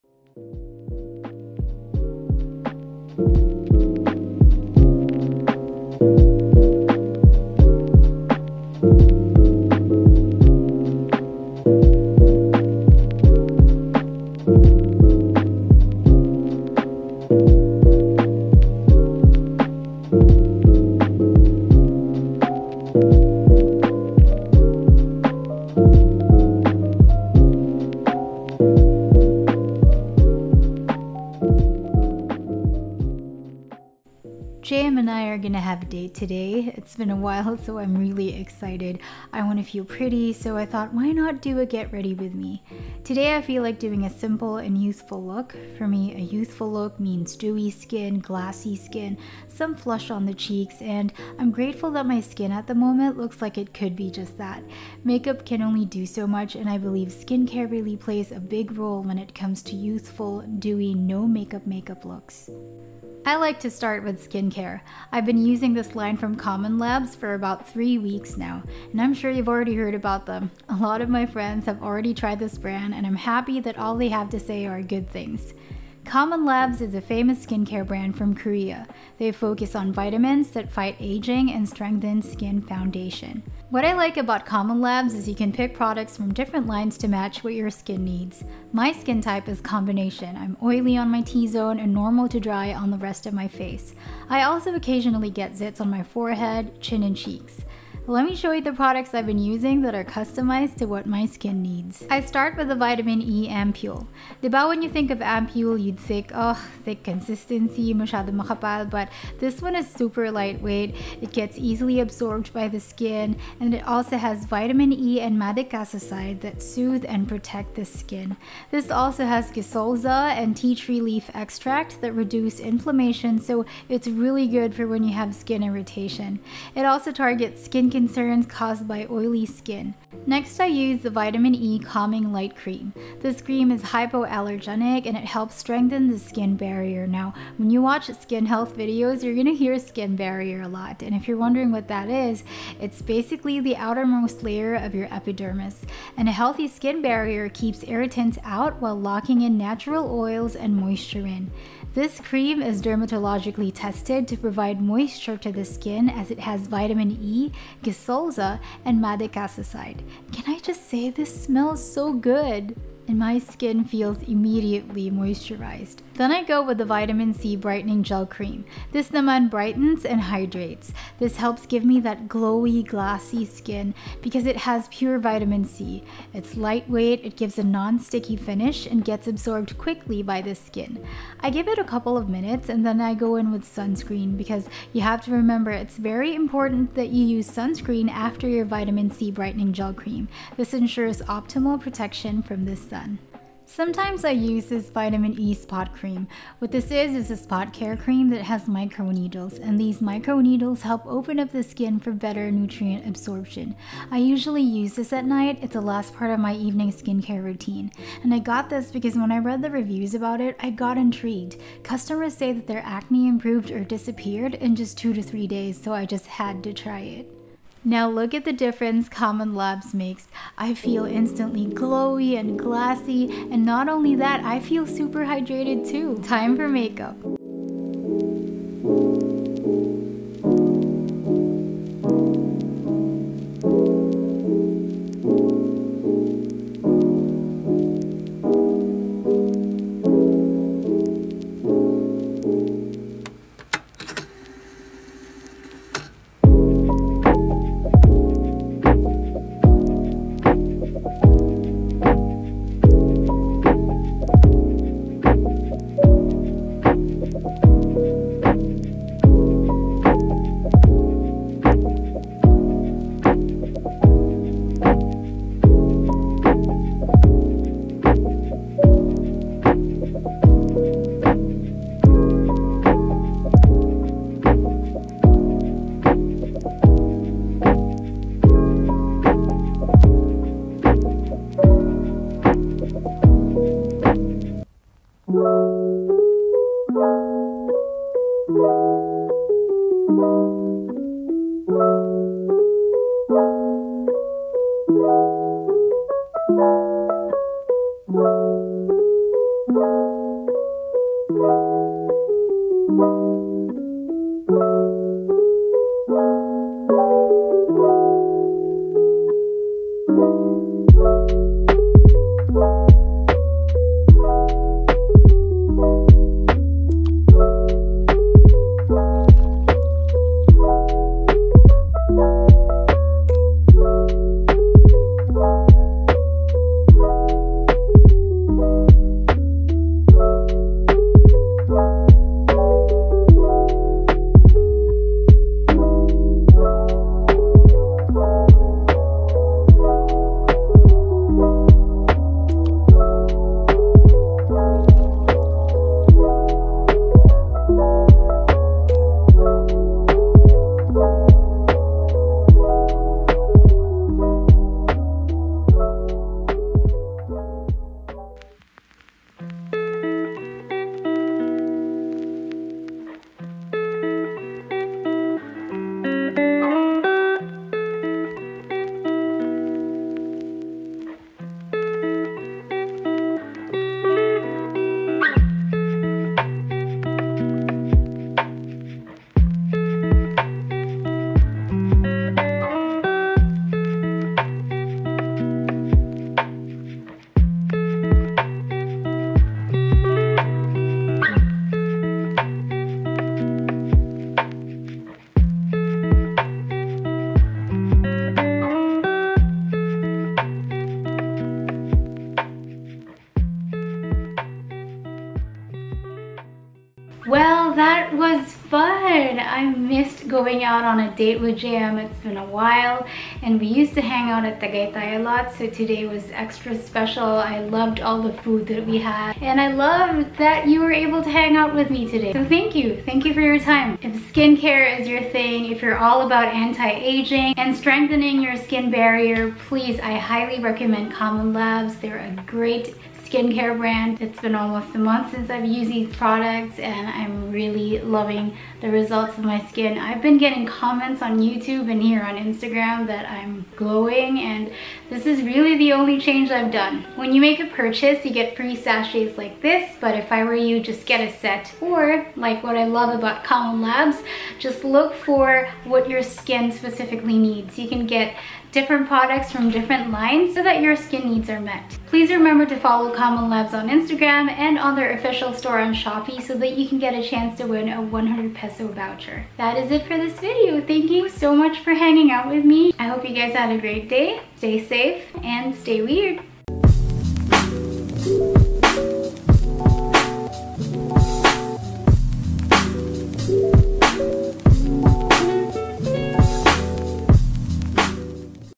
GRWM Glassy Skin for A Romantic Date!
It's been a long time since JM and I went out for a date so I really was really excited for this.
I decided to go with a simple glassy and youthful makeup look. To tell you honestly, I don't think I can don this if not for the skincare routine I've been following in the last few weeks.
Commonlabs is a famous skincare brand from Korea that focuses on vitamins to fight aging and to strengthen skin foundation. I love that you can mix and match products from their different lines to suit your skin needs.

Here are the products I've chosen for my skin type (Combination, oily Tzone, normal to dry on the rest of my face):

1. COMMONLABS Vitamin E Ampoule
2. COMMONLABS Vitamin E Calming Light Cream
3. COMMONLABS Vitamin C Brightening Gel Cream
4. COMMONLABS Vitamin E Spot Cream